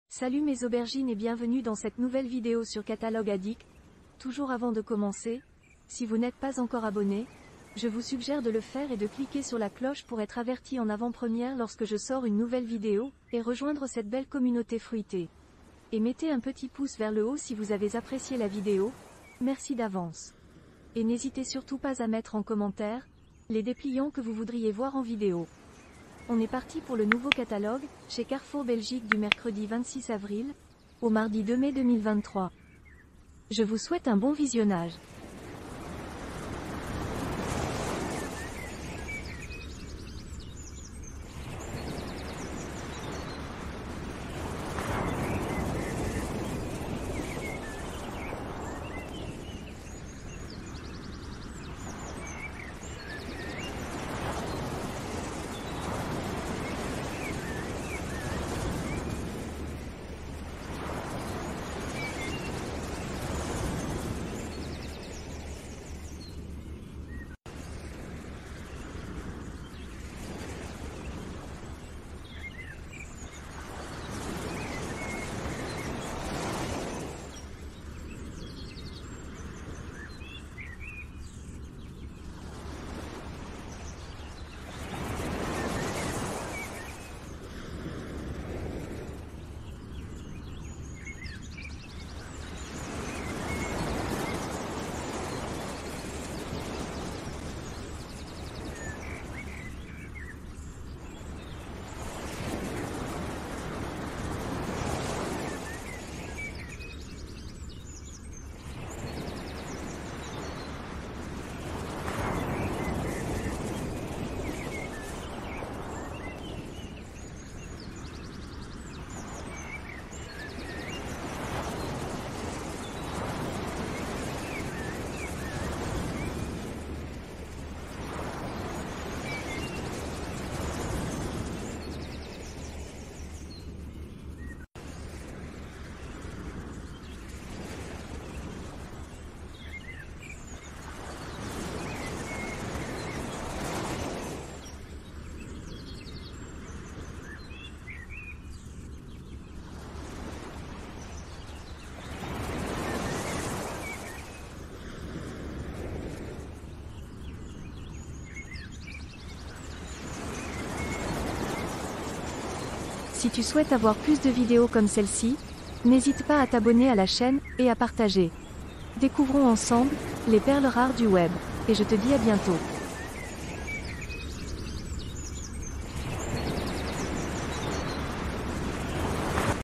folder CARREFOUR du 26 avril au 2 mai 2023 🌞 PROMOS HYPER EXCLUSIVES - BELGIQUE
🛒🛒NOUVEAU folder chez 📍CARREFOUR📍 du mercredi 26 avril au mardi 2 mai 2023 en BELGIQUE!